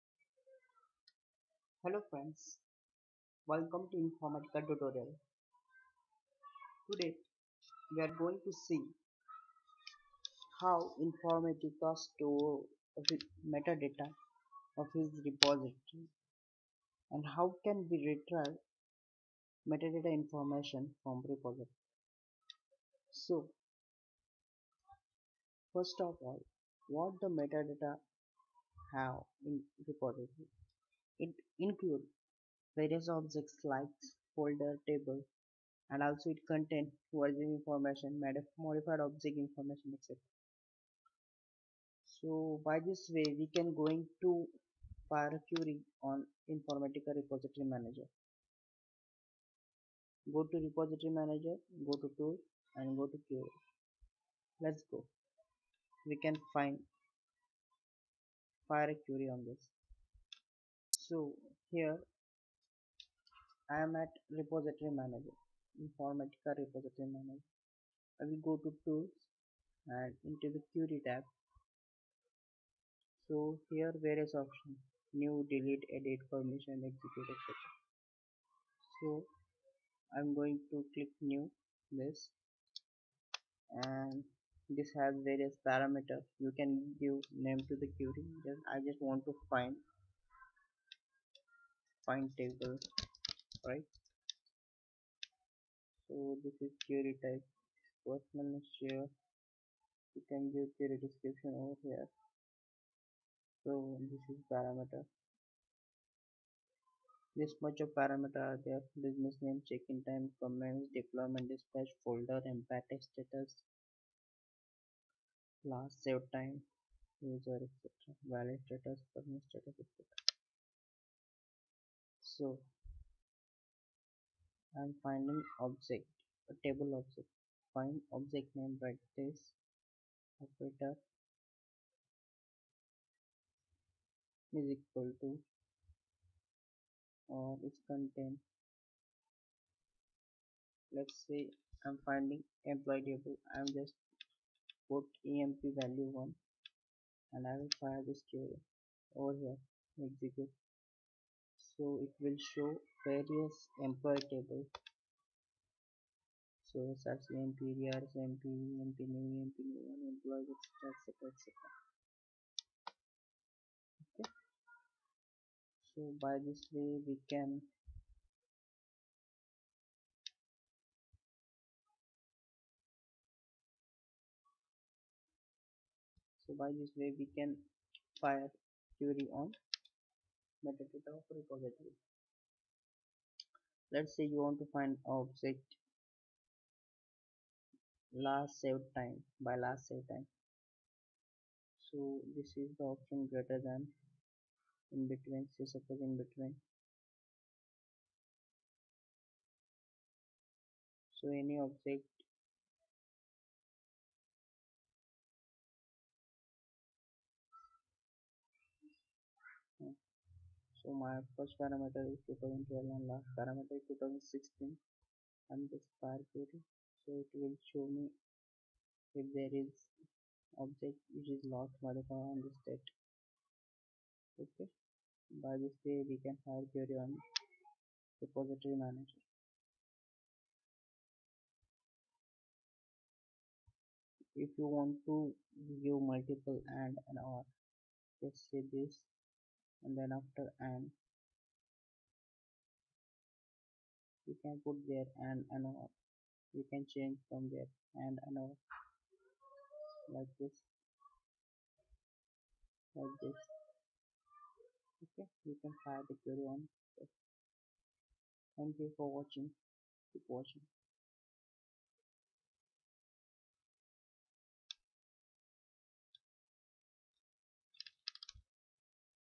Querying the Informatica Powercenter Repository
Informatica meta data include various objects like folder,tables.
Also, it contents version and modified objects information
Follow the Steps to Fire Query Against MetaData:
Repository Manager  - Tools  - Queries. Querying the informatica Powecenter Repository